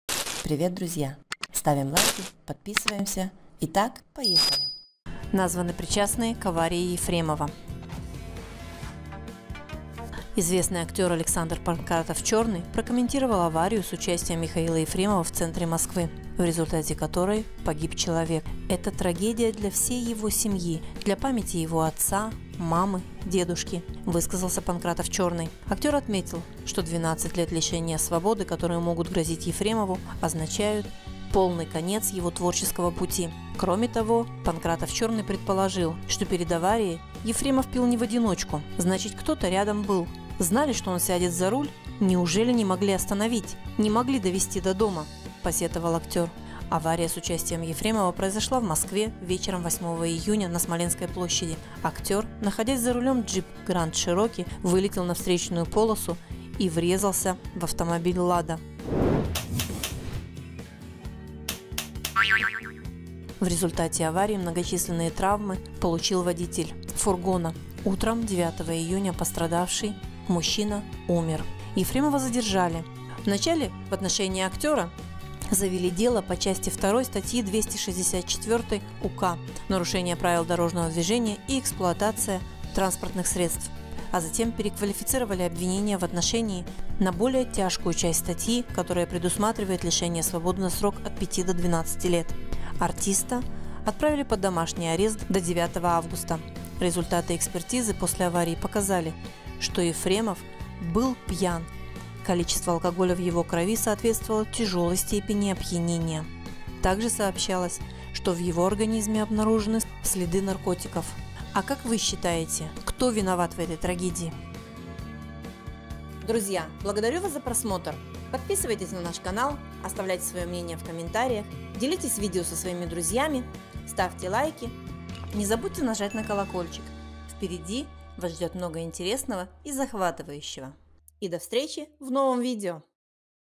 🔥Вскрылась ПРАВДА! 🔥Названы ПРИЧАСТНЫЕ к аварии Ефремова!
Актер Александр Панкратов-Черный заявил, что устроивший пьяное ДТП в Москве Михаил Ефремов «точно был не один».